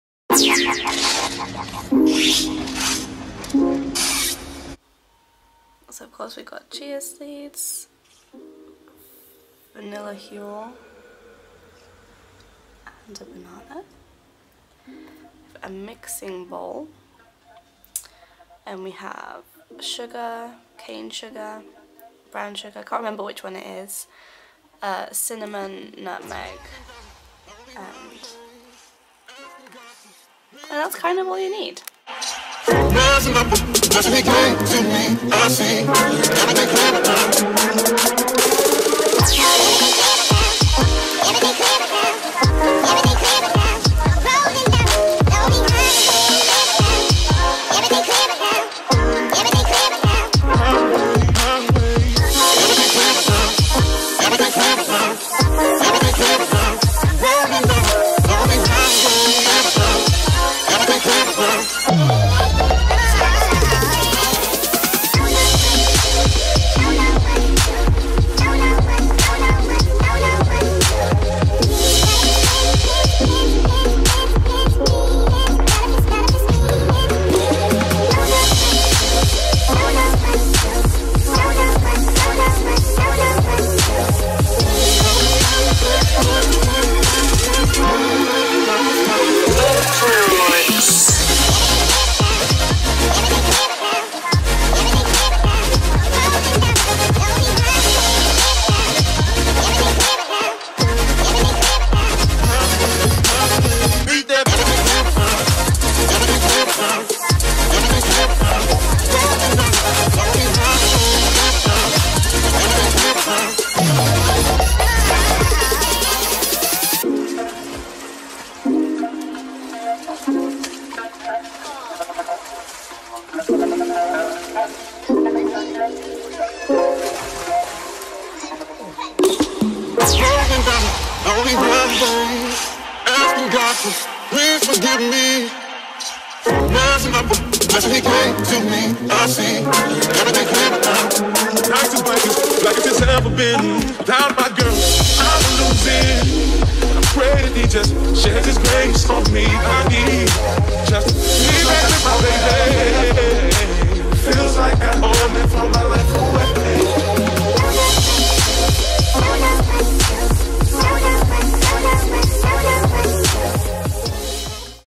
Winter Spiced Huel Mugcake | Vegan
Hey guys!

Another mugcake recipe for you! This one is perfect to warm you up from the inside out when it's so cold outside.

Huel's website if you want to try out the same recipe as me: huel.com

If not, I'd suggest using oat flour, and a vanilla flavouring/syrup.

If you attempt to make this mugcake, please feel free to tag me on Instagram. I'd love to see your creations!

Go vegan!